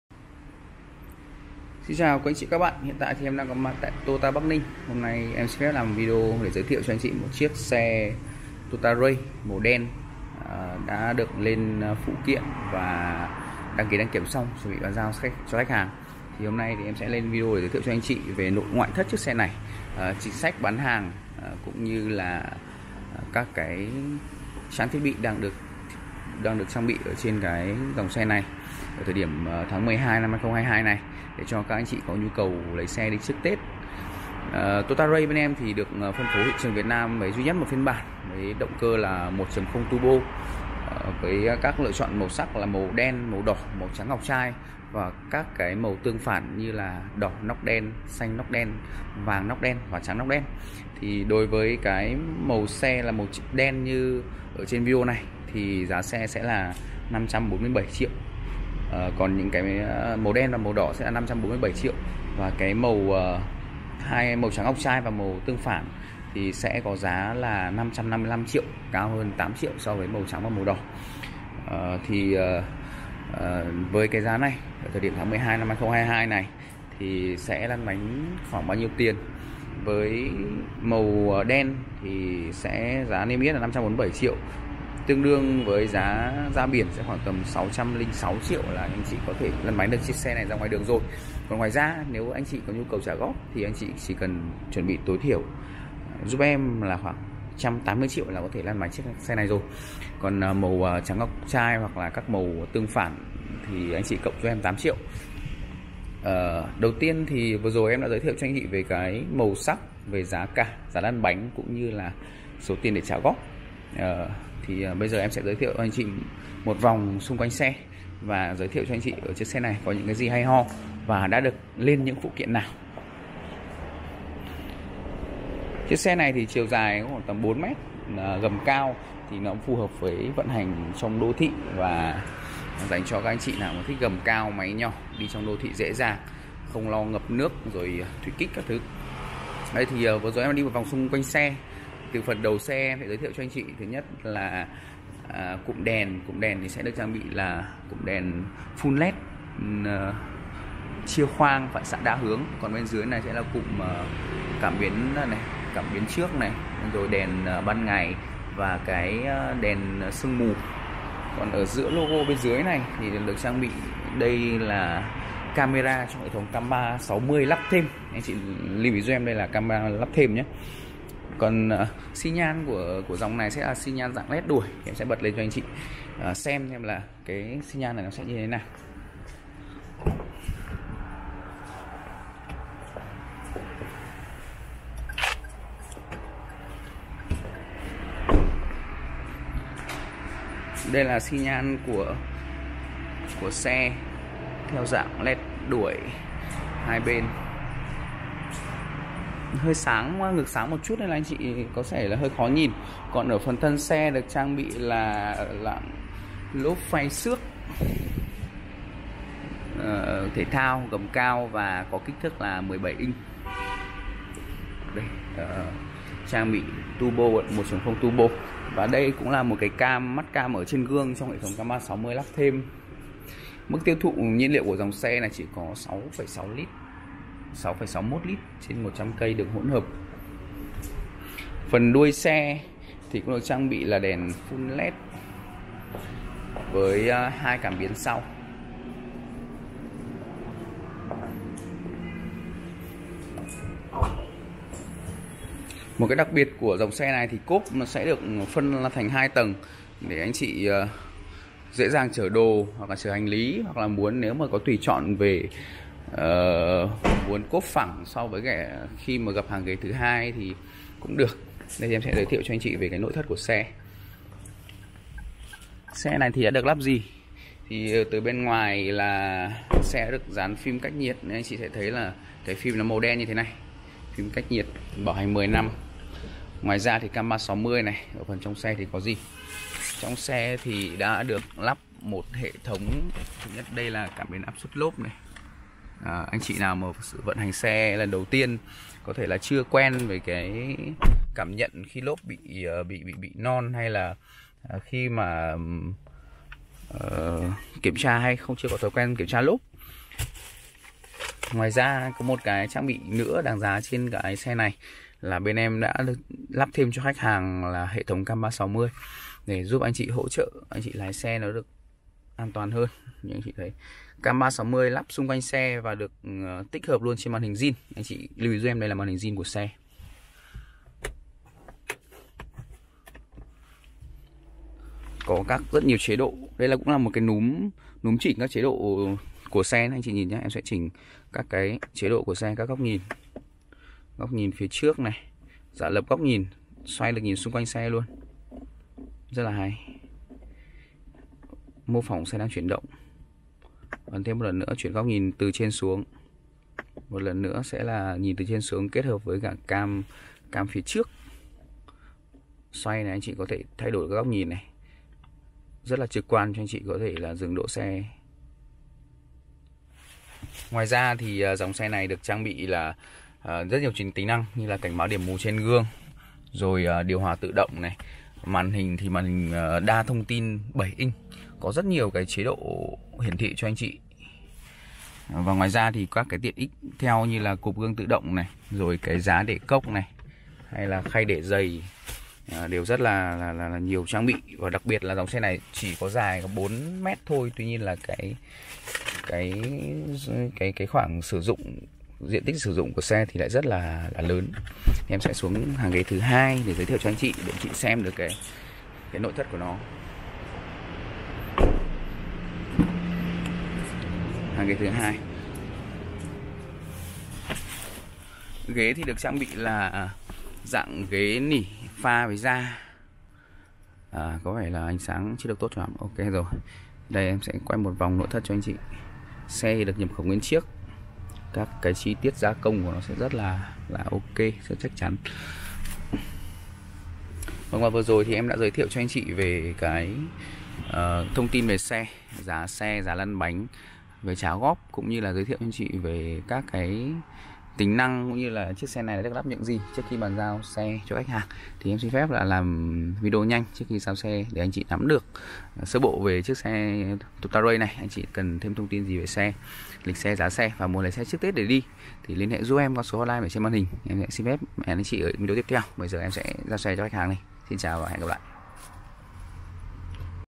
Giá Xe Toyota Raize Khuyến Mại Tháng 12 - 2022 | Tổng Lăn Bánh Tại Bắc Ninh | Raize Nên "ĐỘ" Gì?
+ Hotline Tư Vấn: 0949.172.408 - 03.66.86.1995